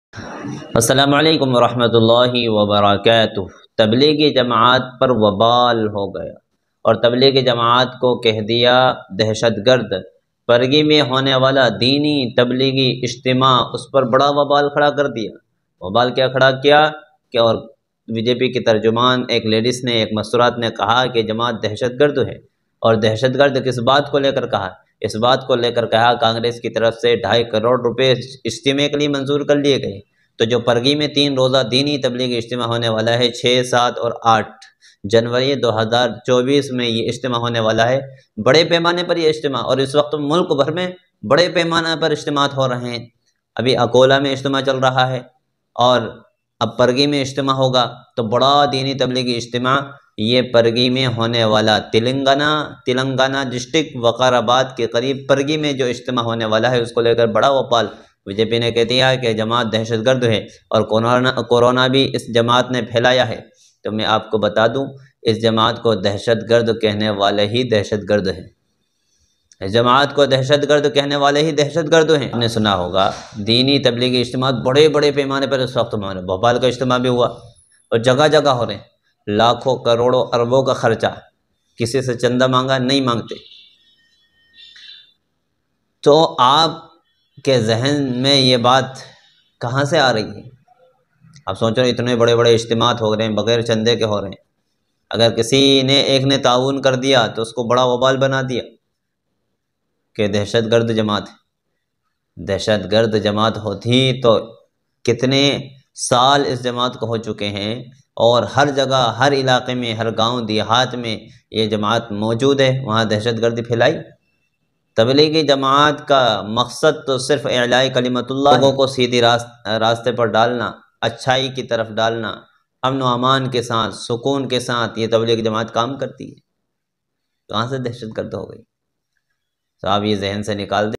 तबलीगी इज्तिमा पर बवाल | parghi ijtima के लिए दिये ढाई करोड़ रुपए
parghi ijtima 2024
tabligi ijtima 2024